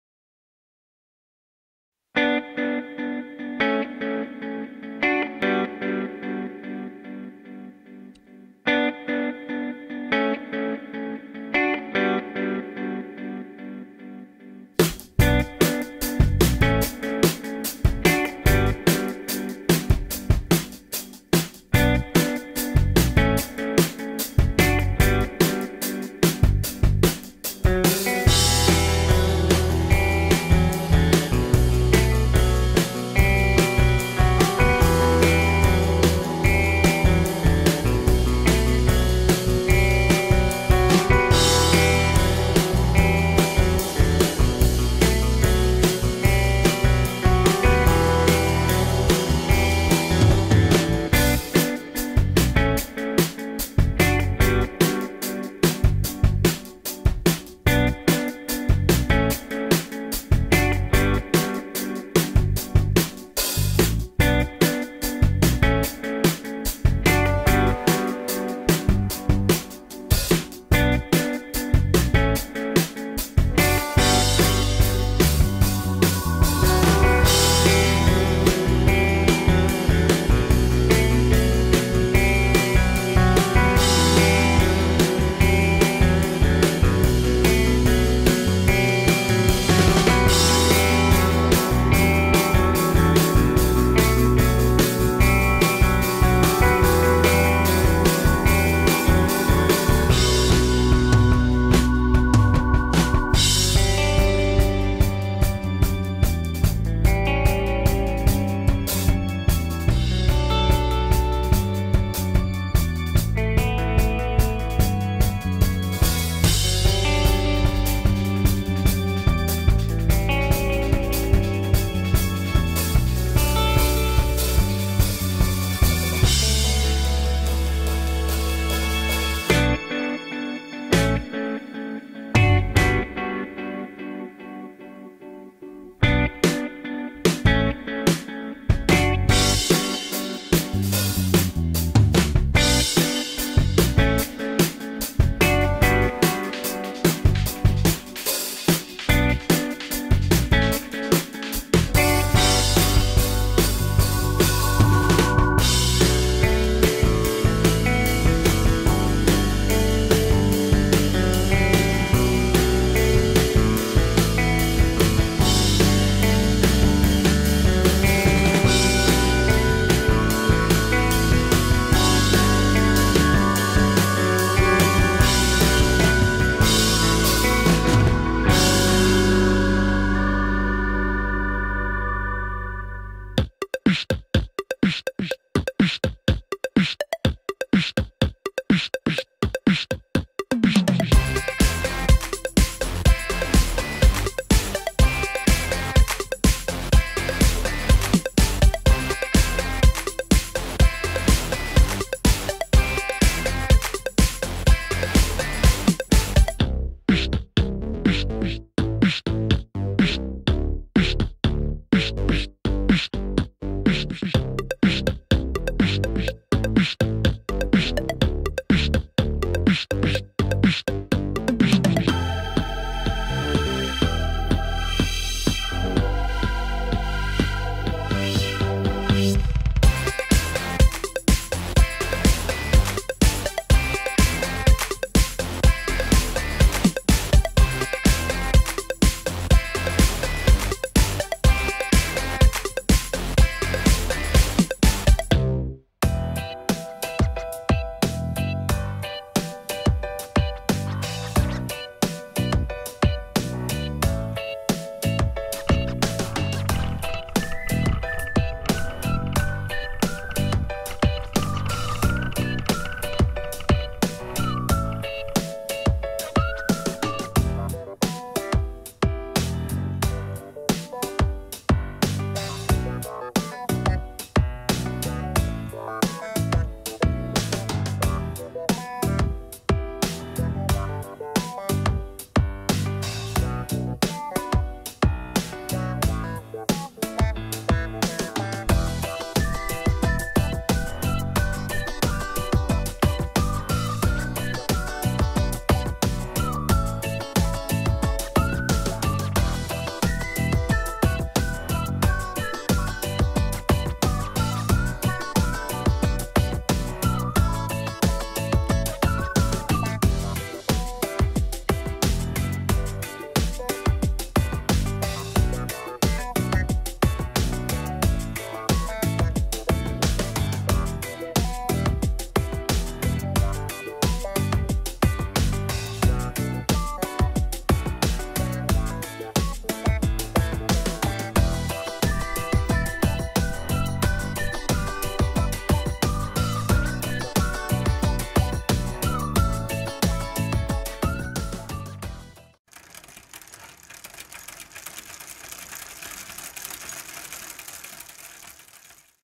Baptist Bottom Brackets 2014 Ride 6
A composite of handlebar video from a number of trails we rode on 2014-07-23 in the Edmonton River Valley and Mill Creek Ravine.

After applying some anti-shake, this is almost watchable :)